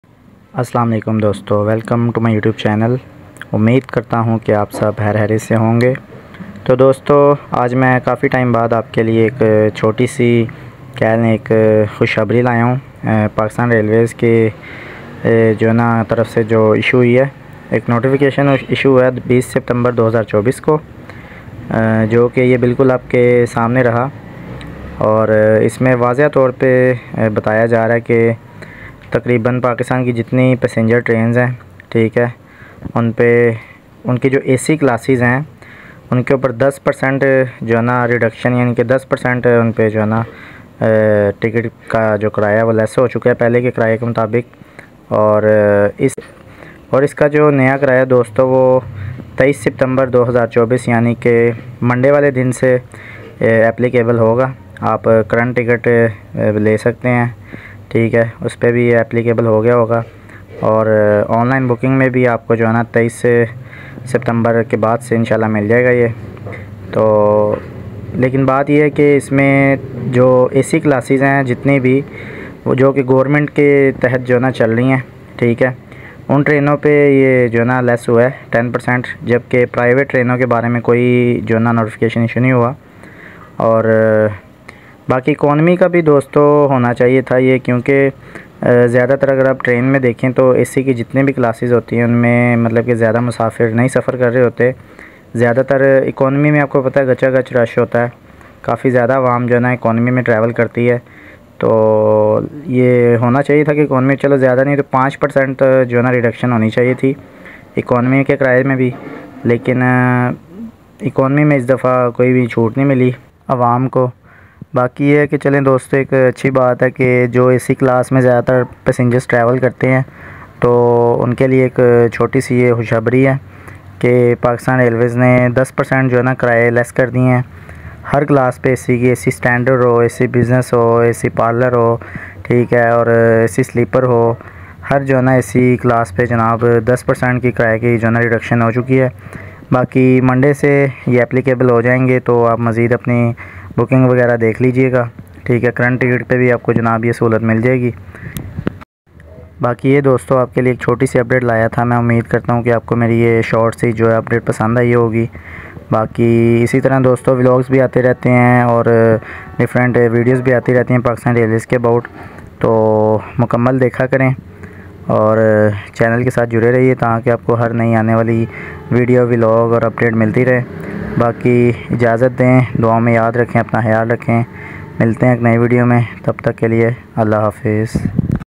Pakistan Railways Less Train Fares Update
This video is about Latest train updates that a new update is confirmed Pakistan Railways 10 percent reduction in fare of all ac classes of and only passenger trains